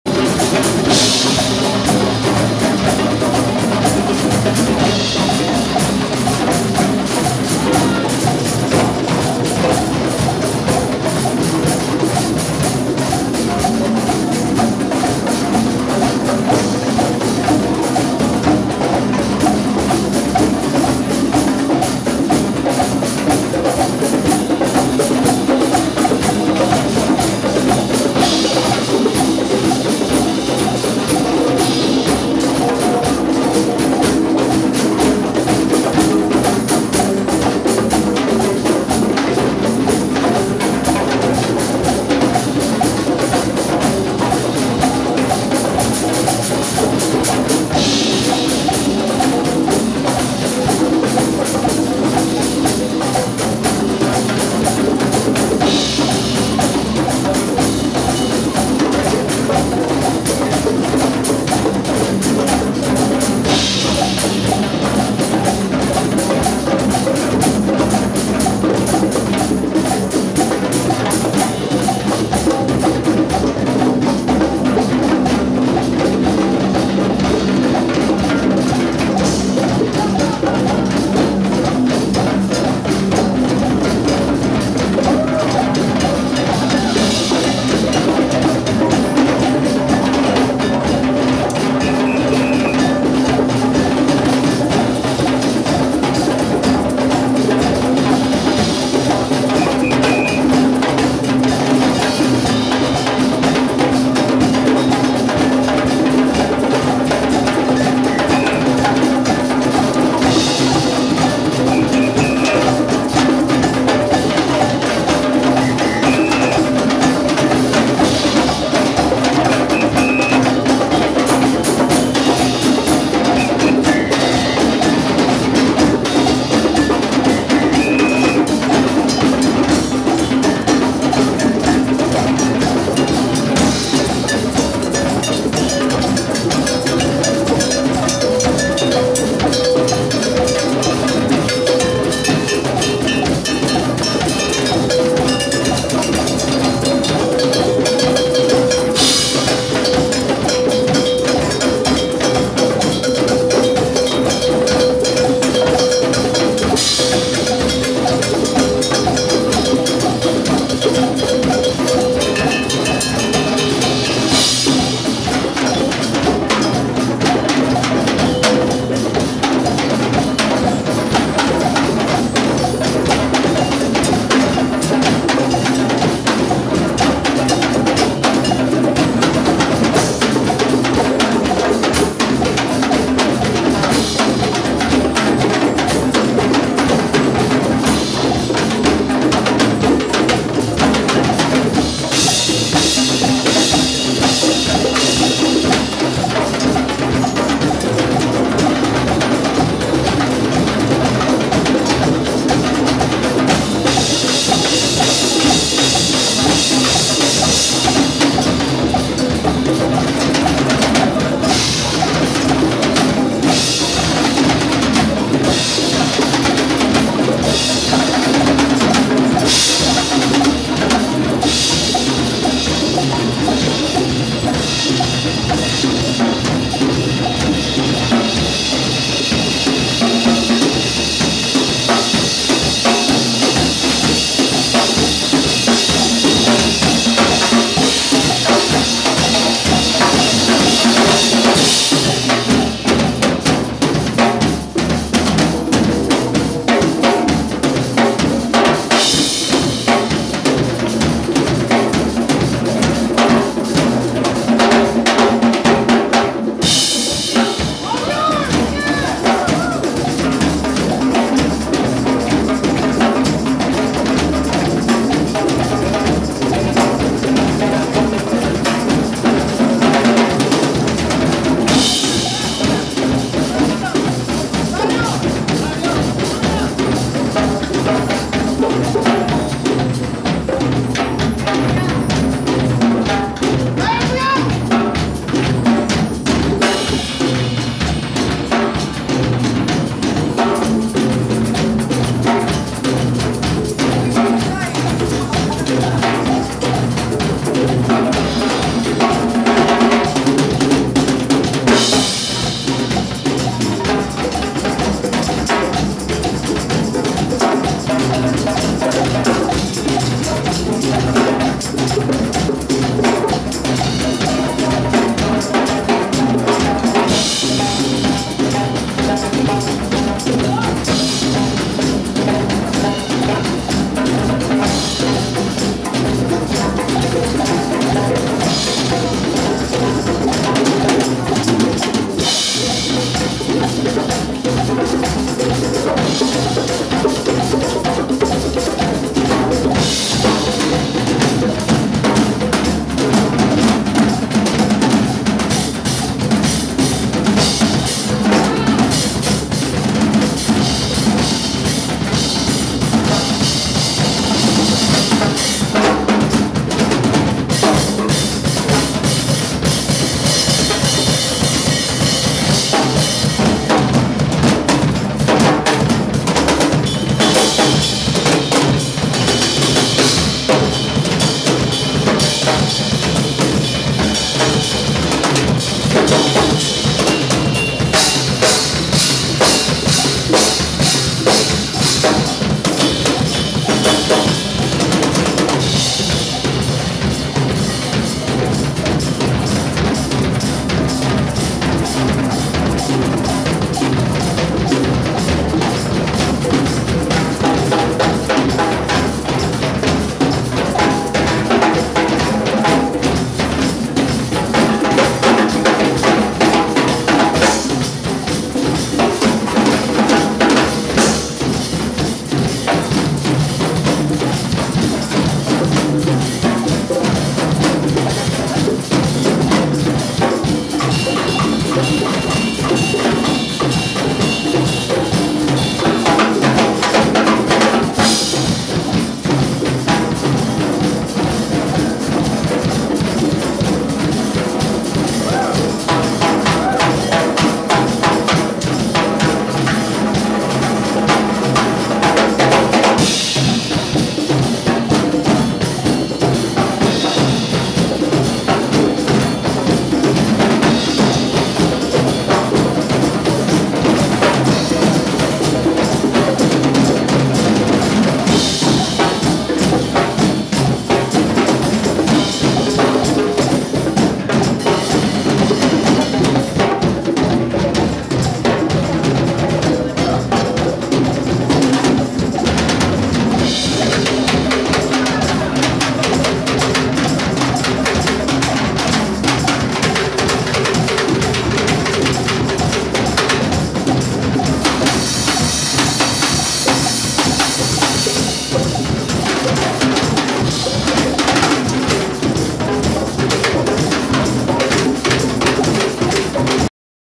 Drum and percussion circle (Drum Day at Lyric)
Percussionists and drummers take part in a collective performance.

Performed at Drum Day, August 22, 2009, at the Lyric music store.